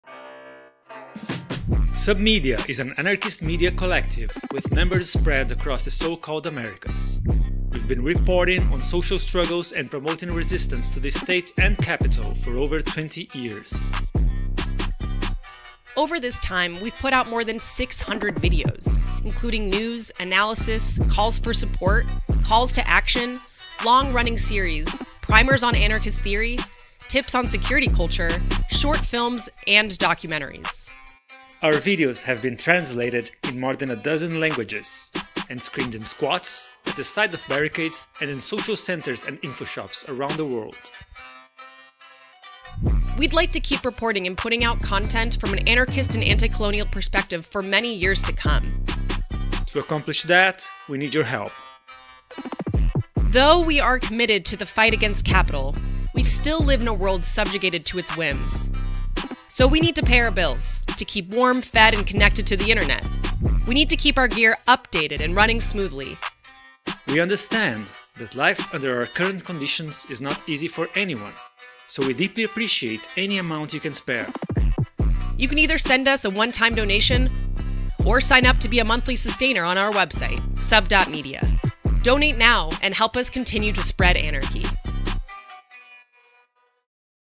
Support subMedia
We at subMedia need your help. For the past 20 years, we’ve been reporting on anarchist and anticolonial struggles around the world. We absolutely could not have done this without generous contributions from our viewership over the years.

We know times are hard, but if you’re in a position to be able to donate, we could really use the help. We would absolutely love for you to sign up to be a monthly sustainer by going and clicking the “Become a Monthly Sustainer” button. If you’re not in a position to donate, we totally get it, we hope you’ve been enjoying our content lately and thank you for your support and solidarity.

Just because we’ve been around for awhile, doesn’t mean we’re well funded. We’re able to do what we do on a shoestring budget because we love making anarchist content and we believe in the struggles we support. With your help we can continue putting out new videos from an anarchist and anticolonial perspective for many years to come.

To follow up on our fundraising progress and see our new exclusive merch designs, head on over